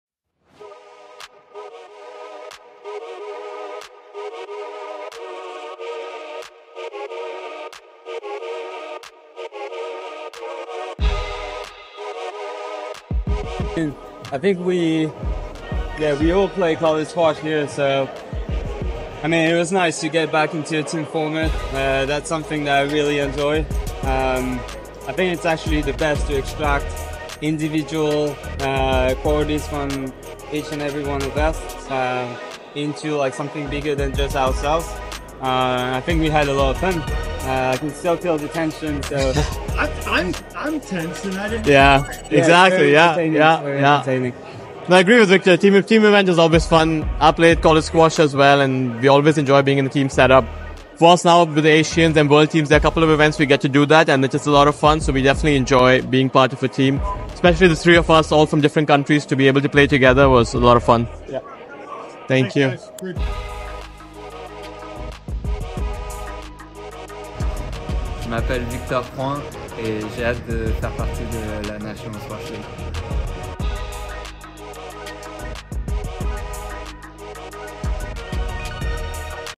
#2 Overall Pick Victor Crouin on the Team Environment in NSL.
Listen to world #11 Victor Crouin and world #38 Ramit Tandon share some thoughts on competing in the team format during NSL Draft Weekend.

Victor was selected #2 overall in the 2024 NSL Draft by the Derby City Colts, and Ramit was selected #5 overall to the Newport Dragons.

Catch the latest updates on our website at nslsport.com